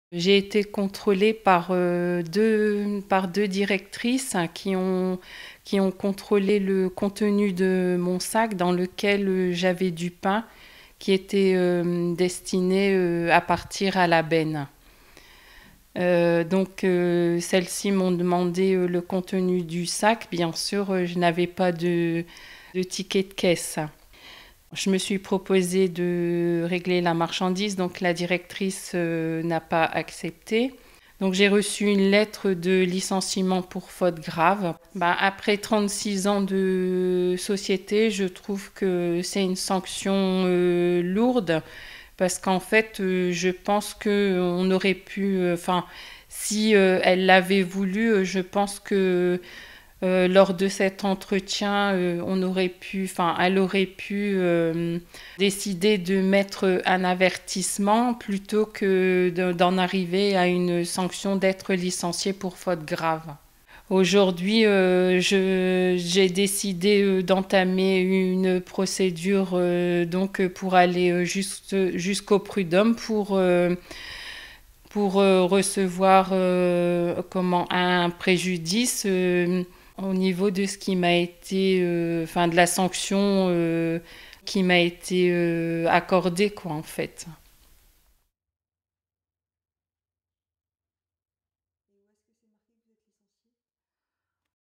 Menacée de licenciement pour «cinq pains, deux baguettes et deux chaussons», elle témoigne
Le 6 mai dernier, alors qu’elle termine sa matinée de travail au supermarché Match de Lille Montebello, Corinne Delacourt est contrôlée par un vigile à la demande de ses supérieurs hiérarchiques. Son sac est fouillé.

A l’intérieur : quatre pains, deux baguettes et deux chaussons au pomme. Des produits que la gestionnaire de caisse dit avoir pris car ils étaient destinés à la benne.

Corinne Delacourt a été priée de ne pas réintégrer son poste dans les jours qui ont suivi. Puis elle a reçu une lettre de licenciement pour faute grave. Une décision qu’elle juge sévère après 36 ans de travail pour cette enseigne. Aujourd’hui, cette habitante de Noyelles-Lès-Seclin espère faire requalifier son licenciement pour faute grave en licenciement abusif devant les Prud’Hommes.

Nous avons contacté la direction du magasin qui ne souhaite pas s’exprimer sur ce licenciement.

Son fils a mis en ligne une pétition sur la plateforme @mesopinions pour soutenir sa mère dans l’affaire de ce licenciement ▶️